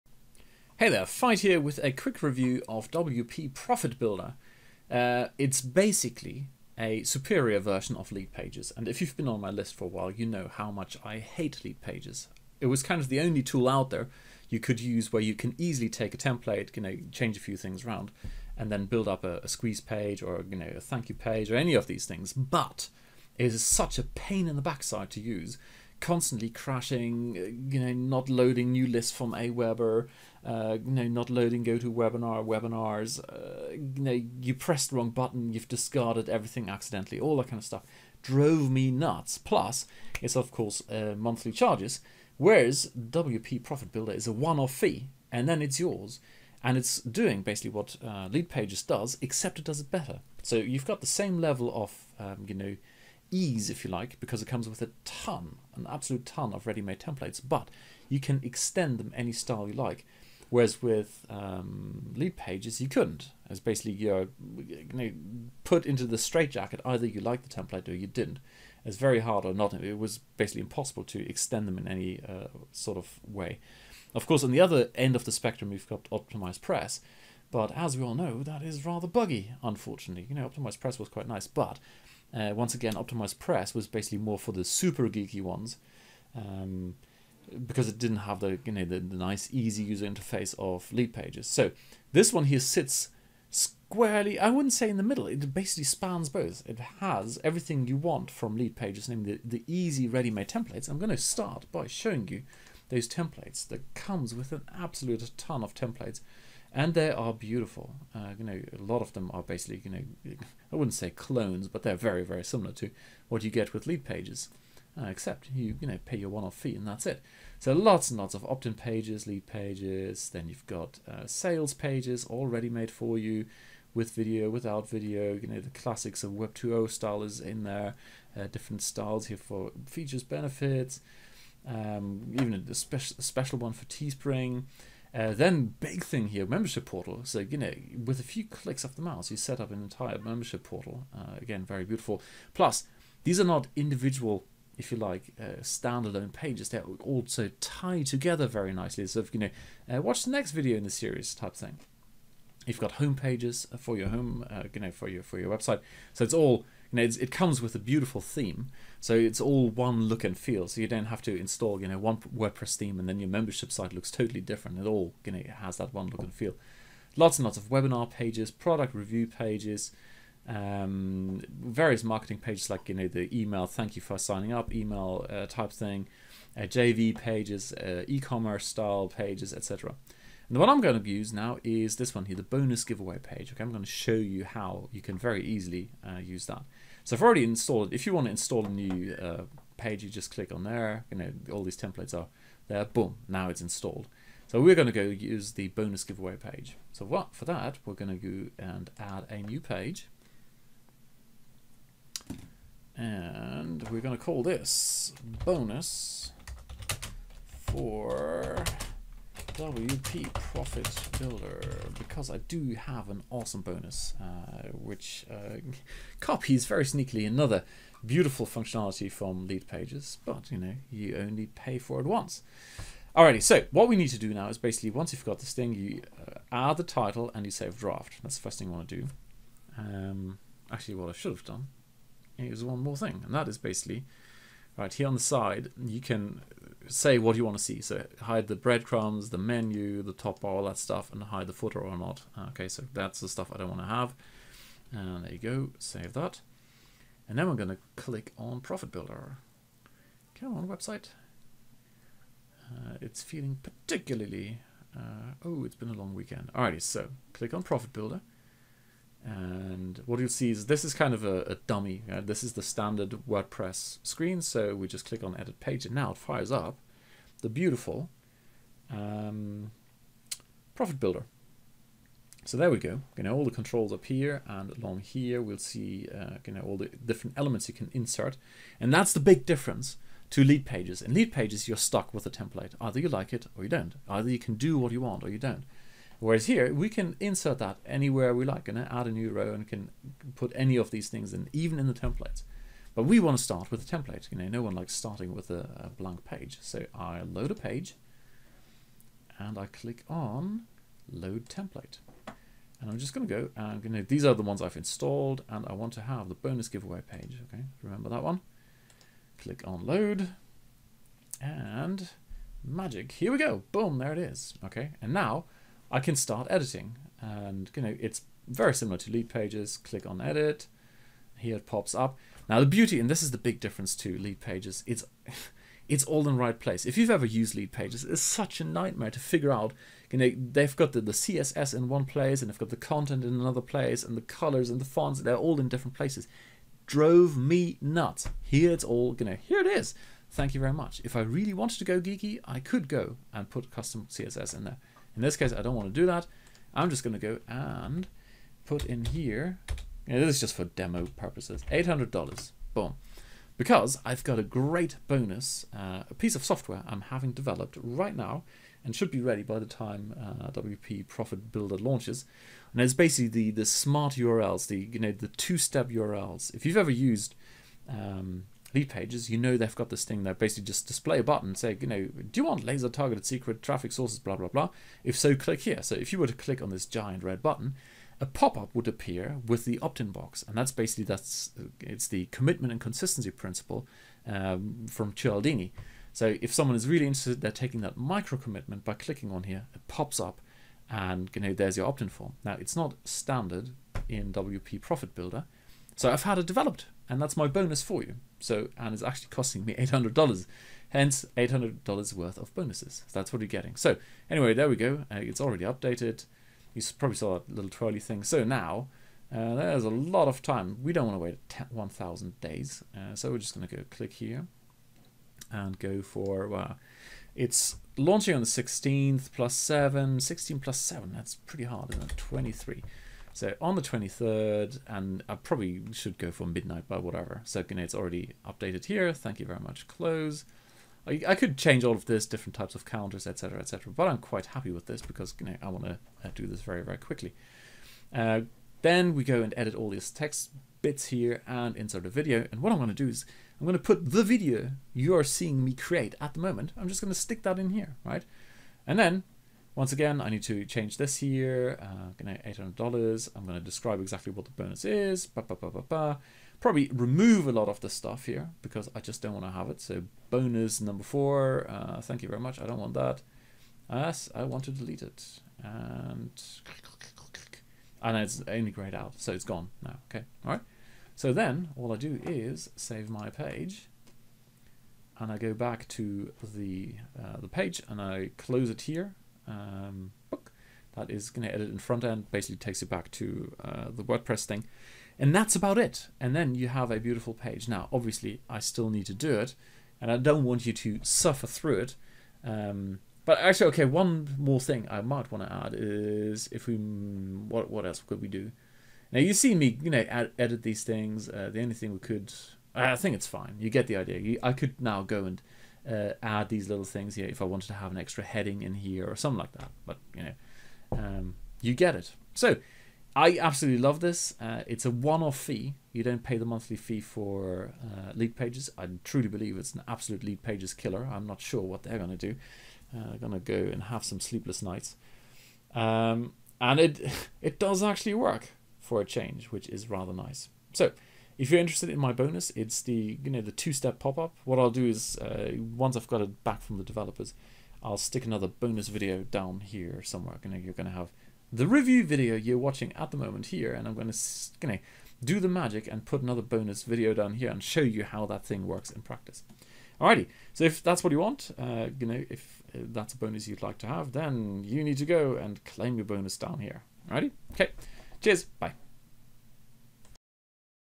WP Profit Builder Review And Bonus
Quick review of WP Profit Builder,along with my bonus offer.

So I had the 2-step signup functionality implemented so you can use it in WP profit builder (and any other site you have: if you can put an HTML link it, you can have 2-step signups;-)